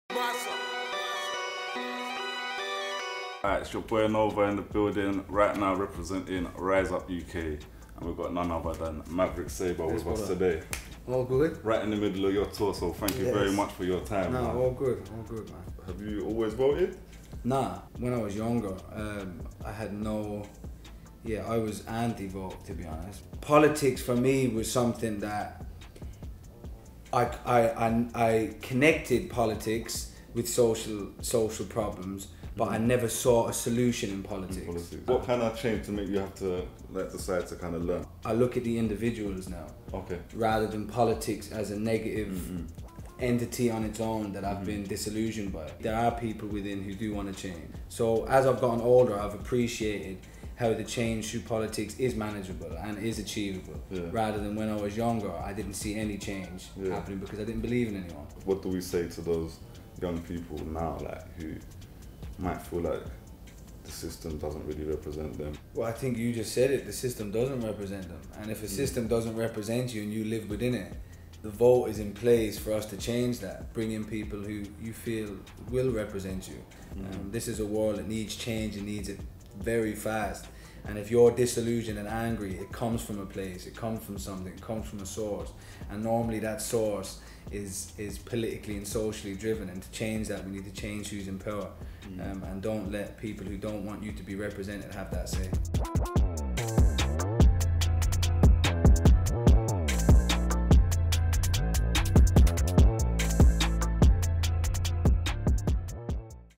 Maverick Sabre talks to Novar FLIP | Rize Up UK
Rize Up UK is a voter registration campaign aimed at young people from marginalised communities. Maverick Sabre sat down with Novar of Flip Life Media to talk about why he supports the campaign and the importance of voting.